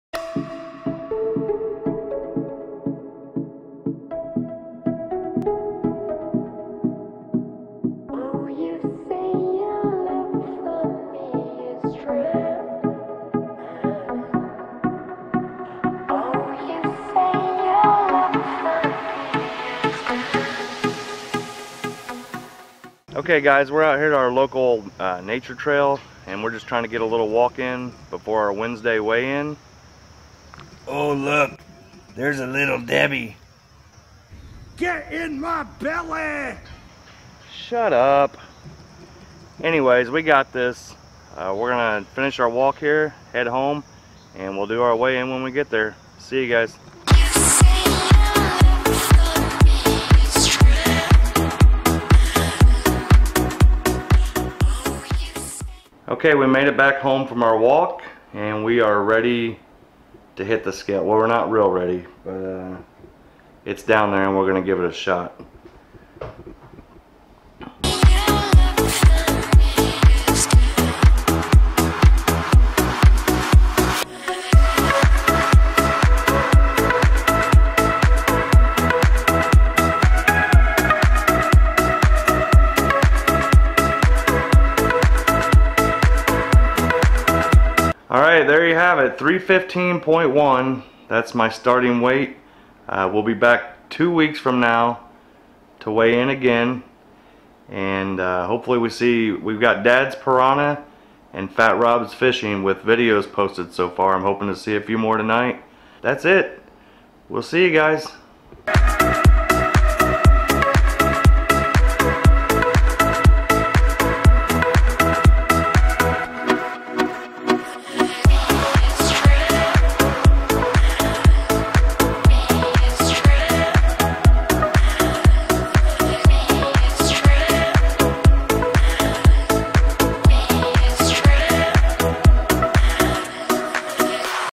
fatanglerchallenge WEIGH-IN WEDNESDAY - with a Special Guest
Weigh-In Wednesday with special guest Fat Bastard!  We are starting at 315 and the goal is to lose weight and get healthy!

fatanglerchallenge VIDEOS posted so far

LOCATION:   Southern, IL

KAYAK EQUIPMENT:

- 2017 Jackson Coosa HD.  Thanks to Beth and Miles @ for getting me into the kayak that worked best.